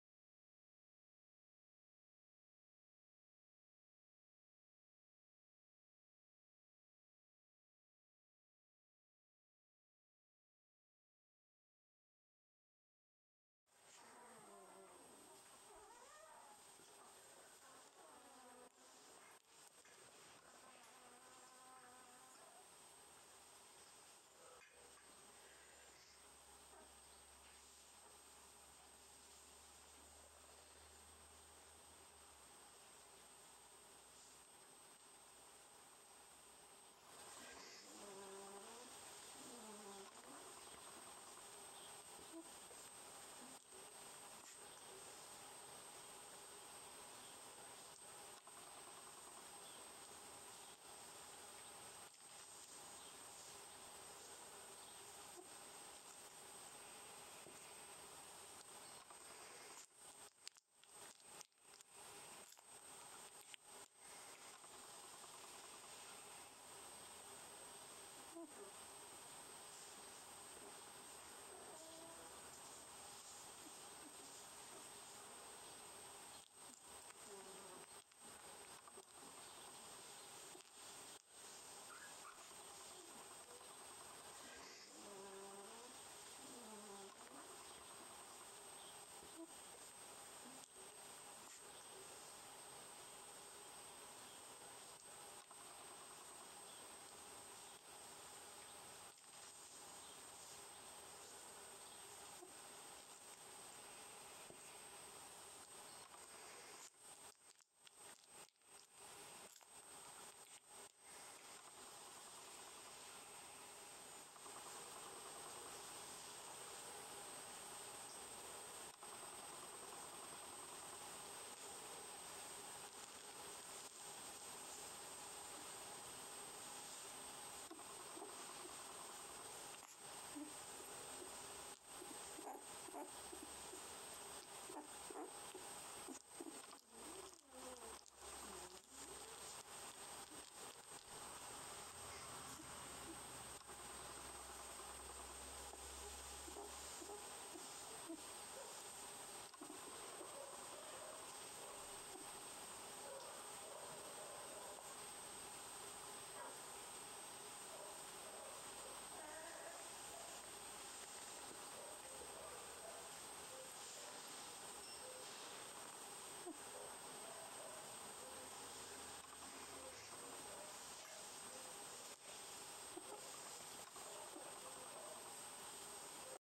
COLLIE rough puppy Owczarek Szkocki śniady piesek 01
Rough collie puppy, dark sable male number 01, 12 days old.
Parents: Ch.SAGA Actis & Multi Ch. Born for Agara and African Gold